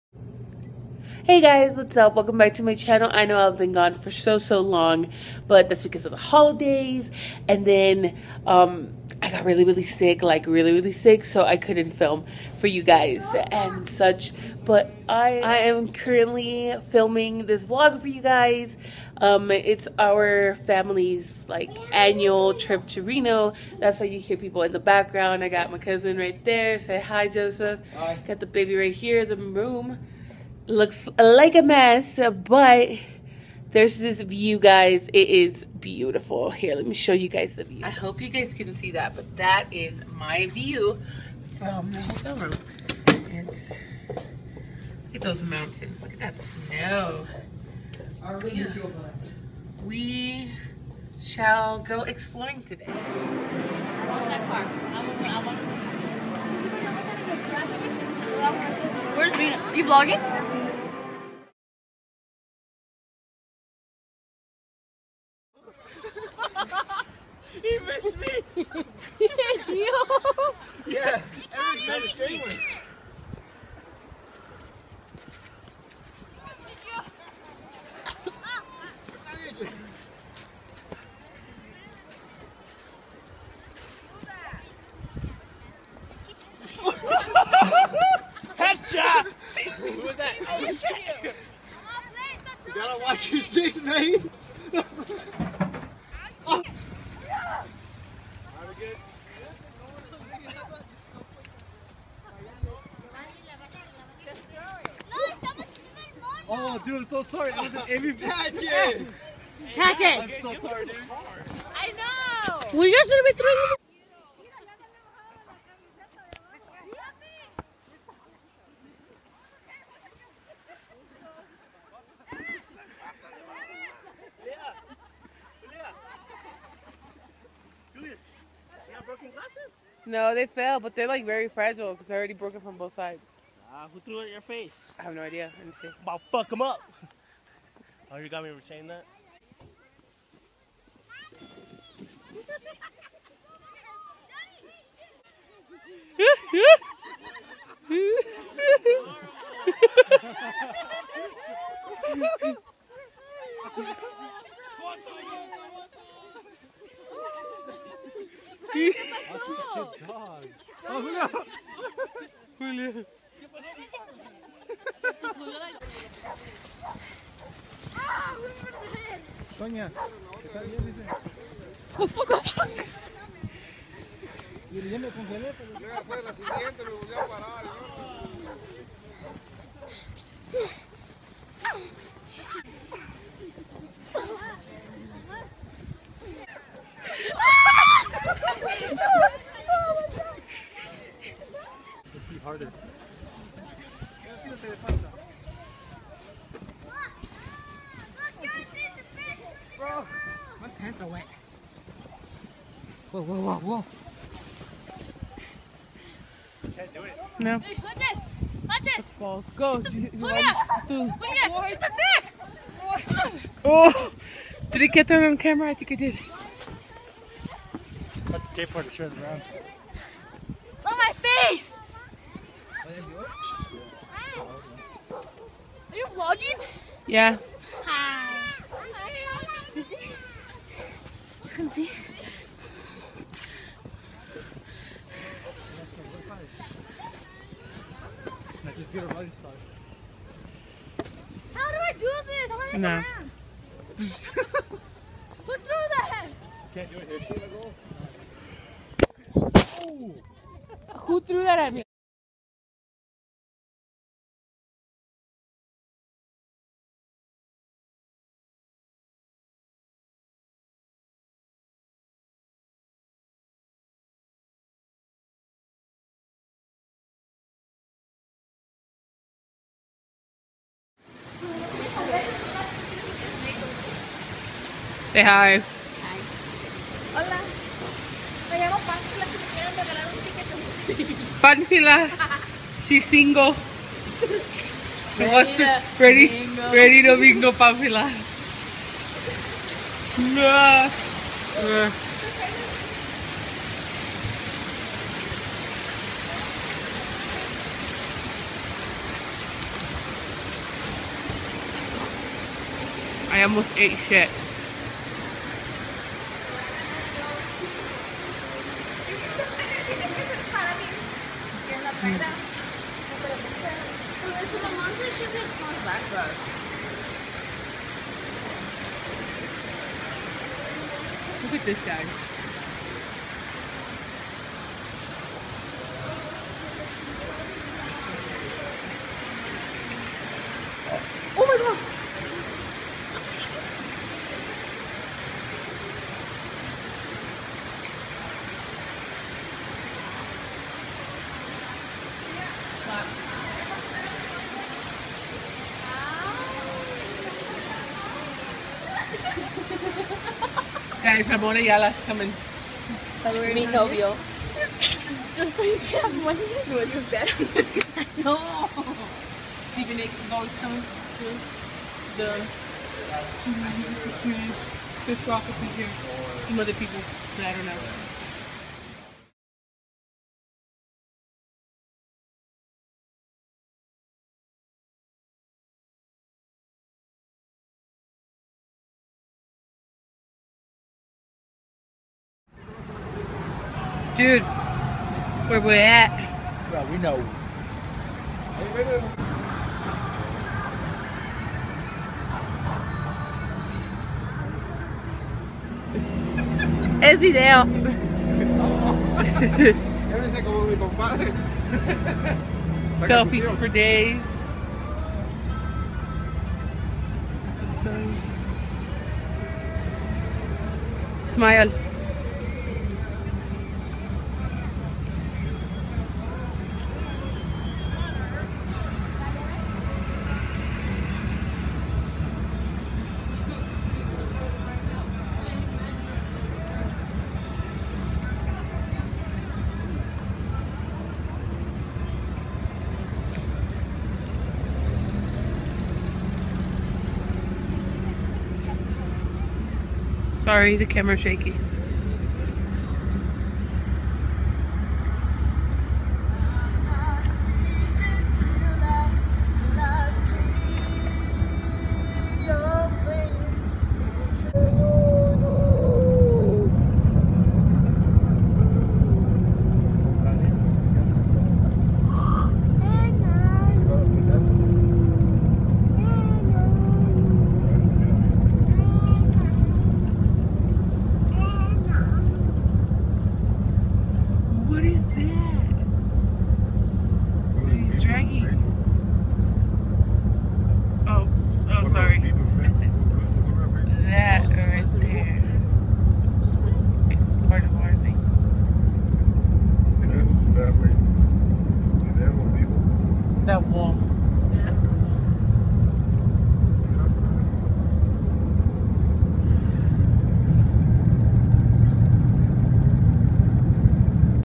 Annual Trip to Reno Vlog
Just showcasing a couple of cool moments from my family reunion